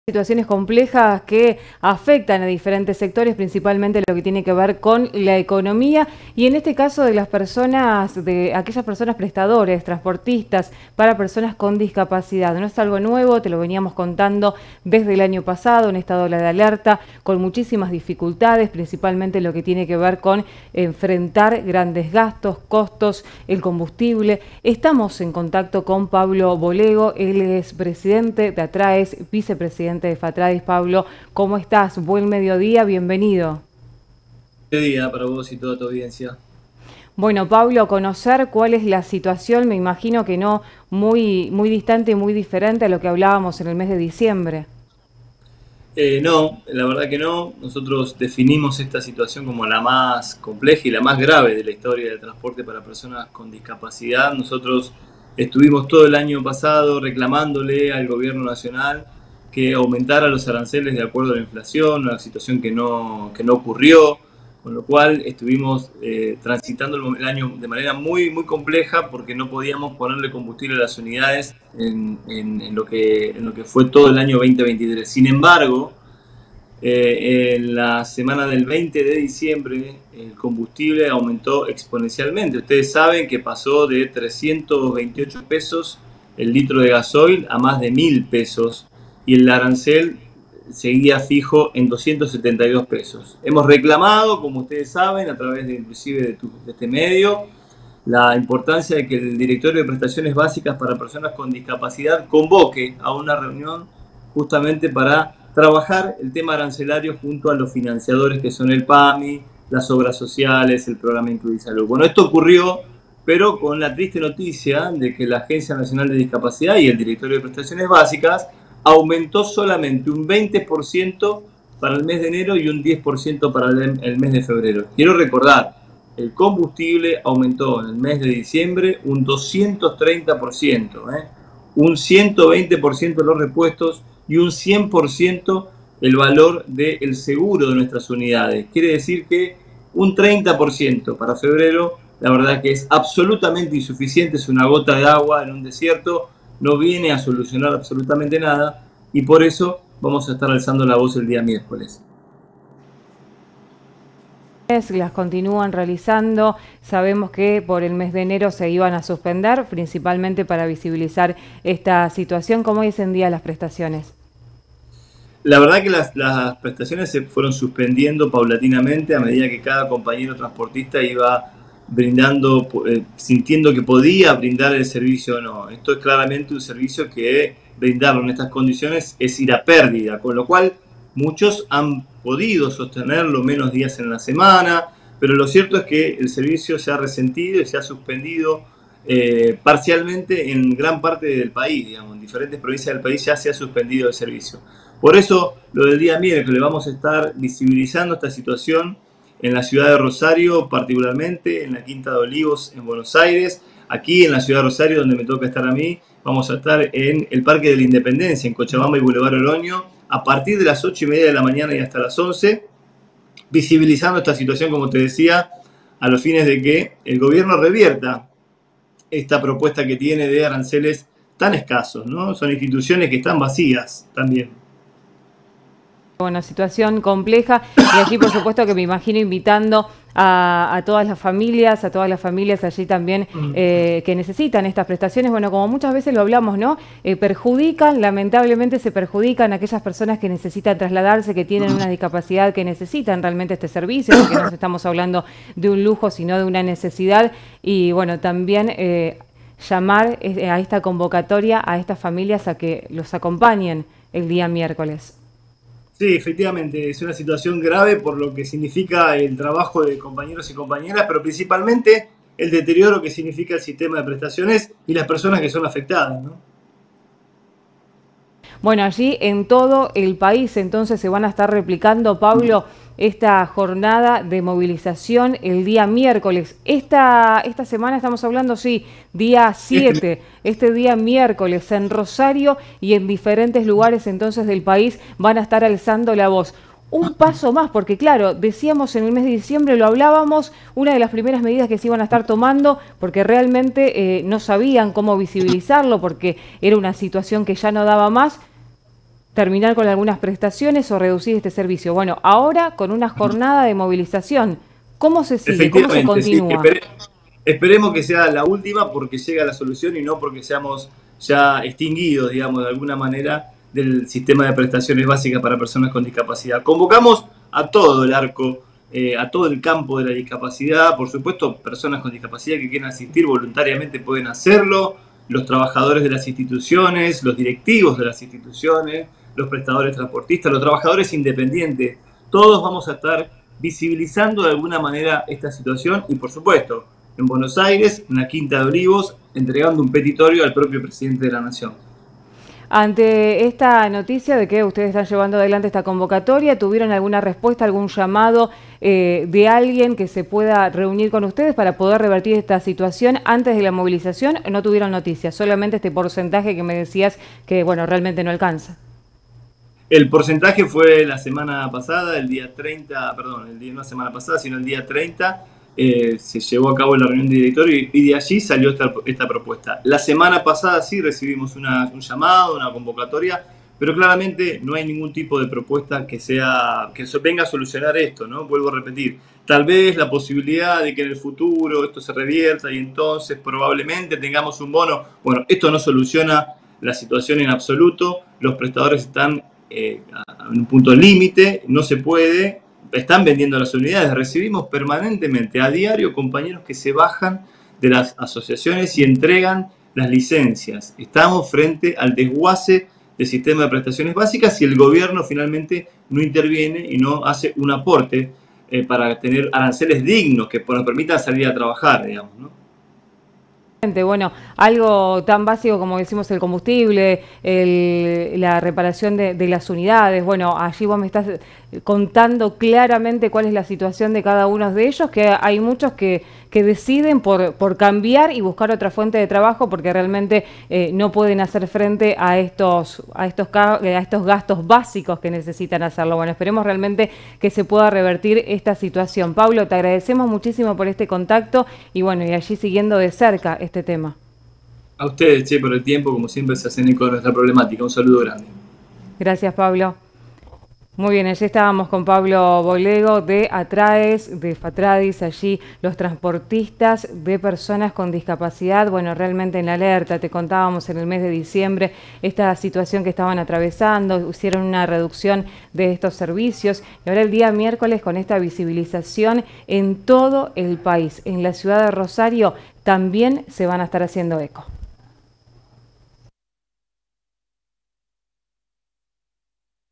Crisis absoluta para los Transportes Especiales
Pablo Bolego, titular de A.TRA.ES., la Asociación de Transportes Especiales de Santa Fe, señaló la profundización de la crisis que sufre este sector y que podría decantar en el cese total de actividades.